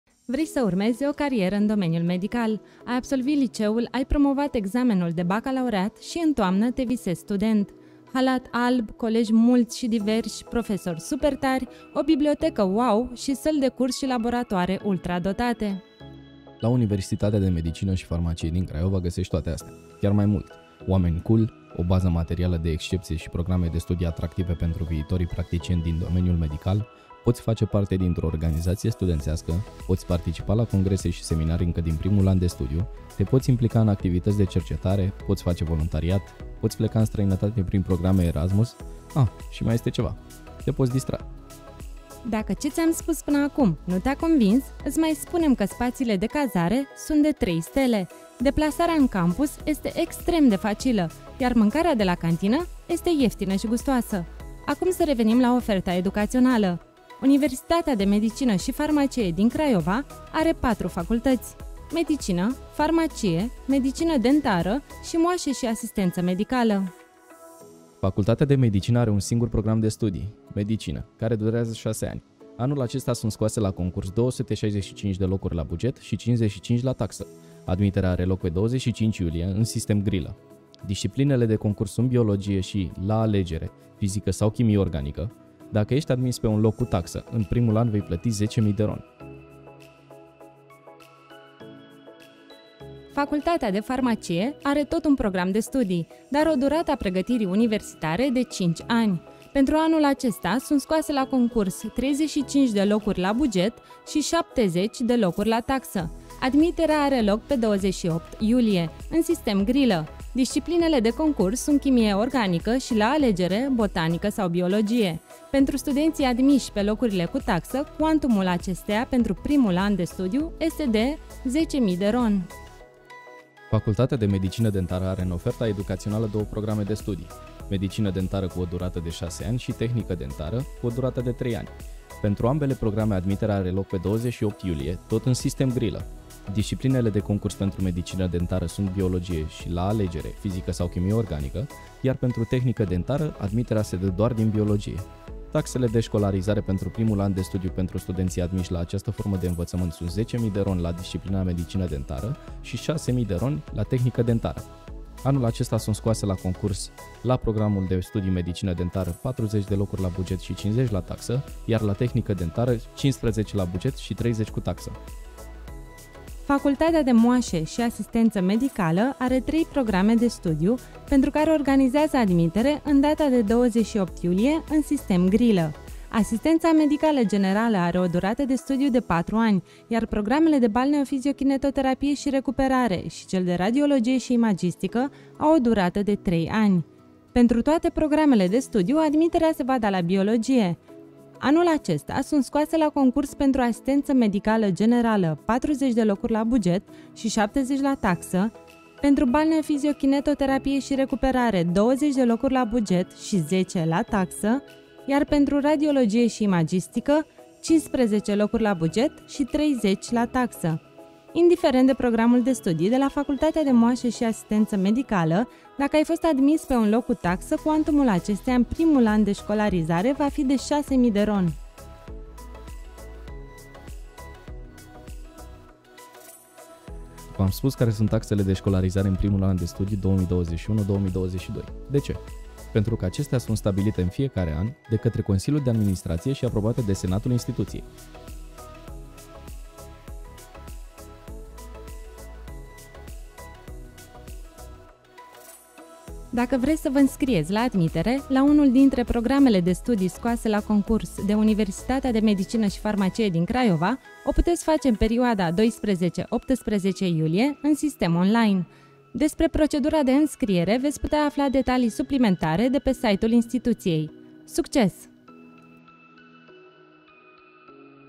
Admitere UMFCv 2021-2022
Oferta educațională a Universității de Medicină și Farmacie din Craiova pentru anul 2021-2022.